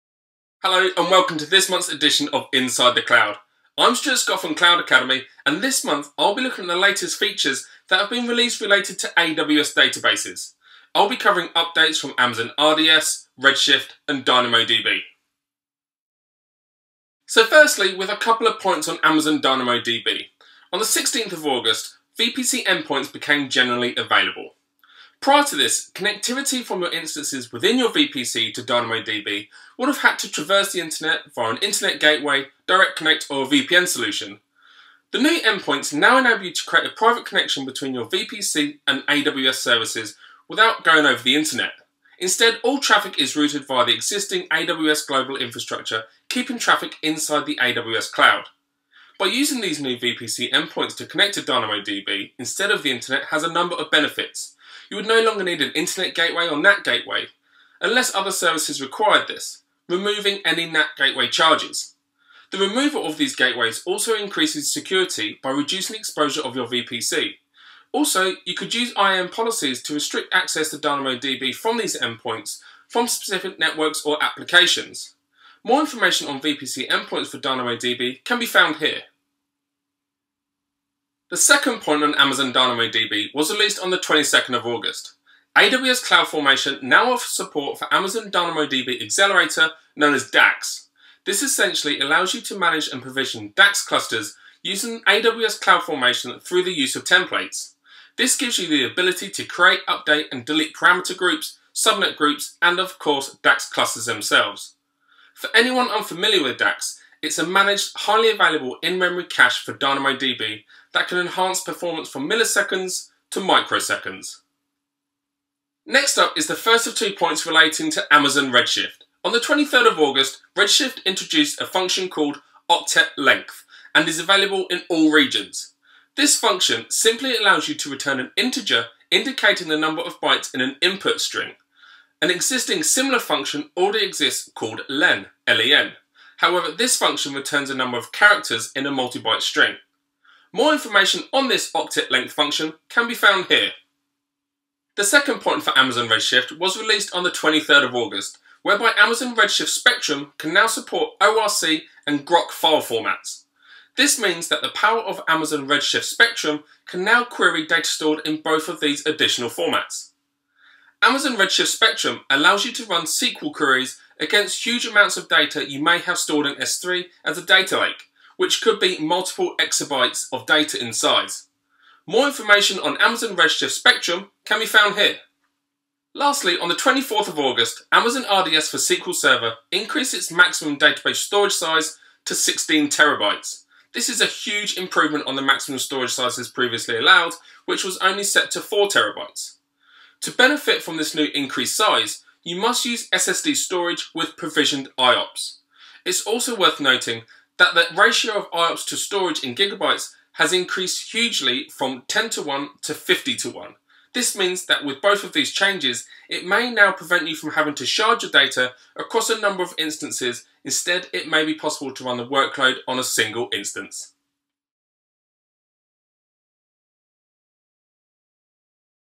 Inside the Cloud - Episode 2: AWS Database News
This month, Inside the Cloud analyzes 5 AWS features announcements on Databases.

Stuart Scott, AWS Trainer and Security Expert at Cloud Academy, looks at 5 different Amazon Web Services updates covering Amazon Relational Database Service (RDS), Redshift, and DynamoDB.